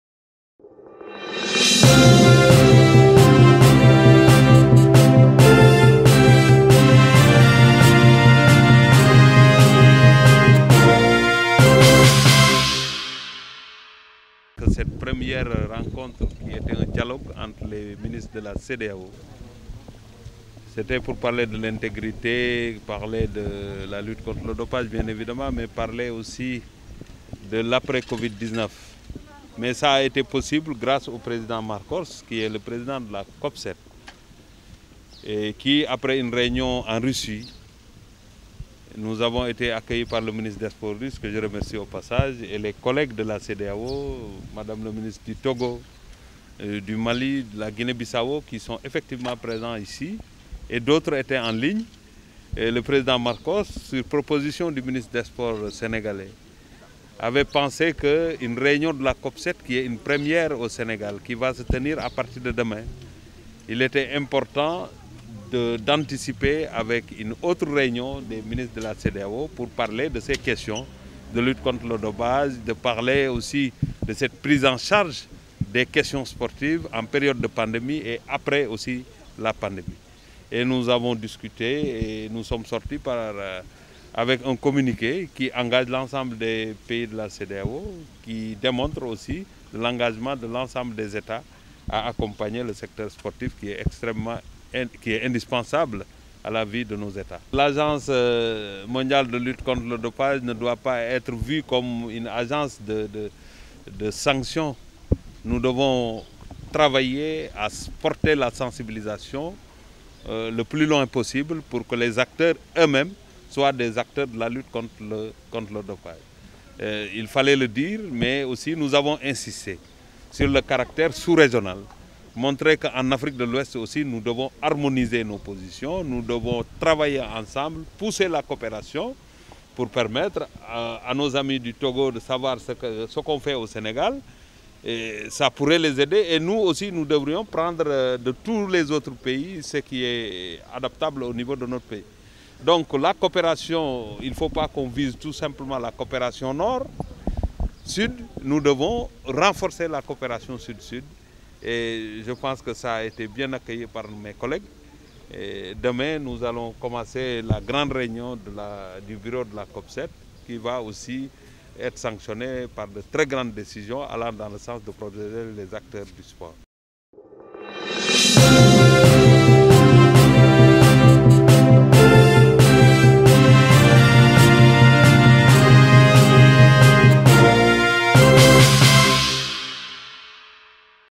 COP7-DIALOGUE MINISTÉRIEL DE HAUT NIVEAU :"RENFORCER L'ÉTHIQUE, L'INTÉGRITÉ, ET LES VALEURS DU......
Matar Ba, hôte des Ministres en charge des Sports de la CEDEAO, pour le renforcement de l'éthique, l'intégrité et les valeurs du Sport après la pandémie de COVID19